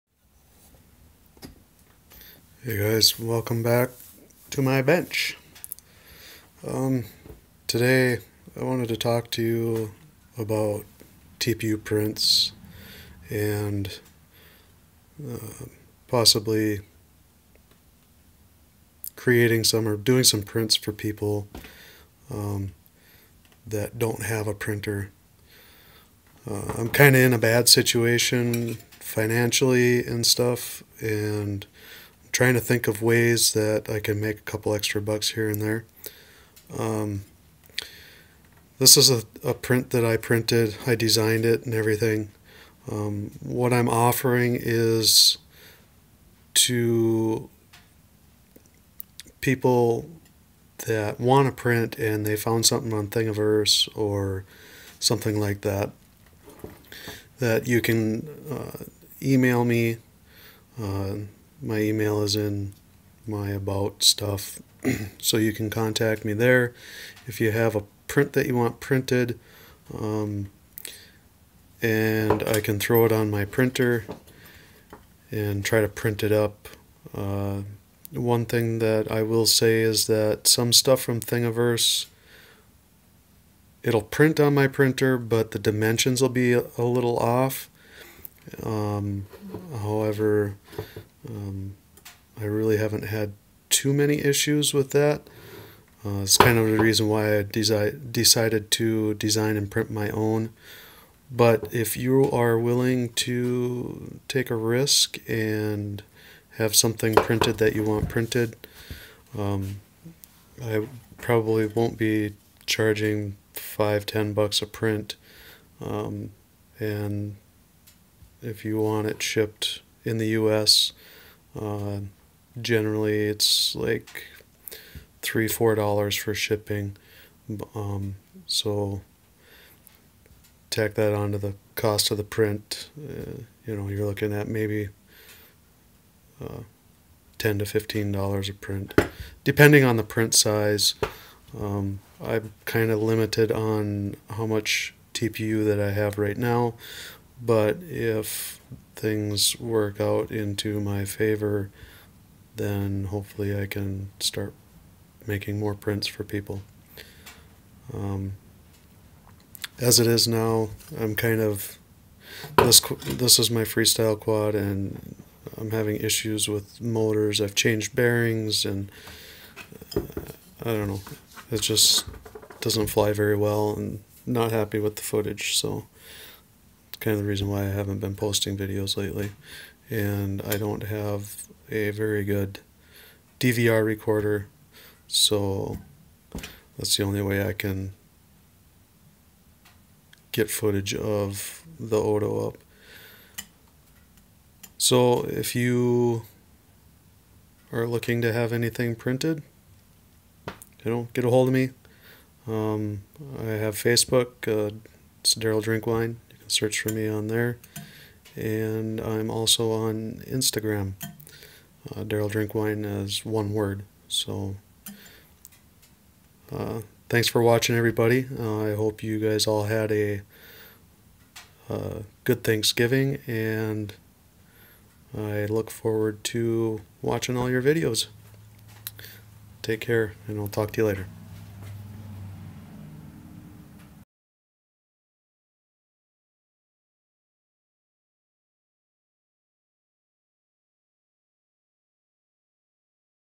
3D Printing Service? - TPU
Have something you want printed, and don't have a printer? Have something you want custom made? I'm offering my services to you. Have something you want custom printed? Let's talk about it.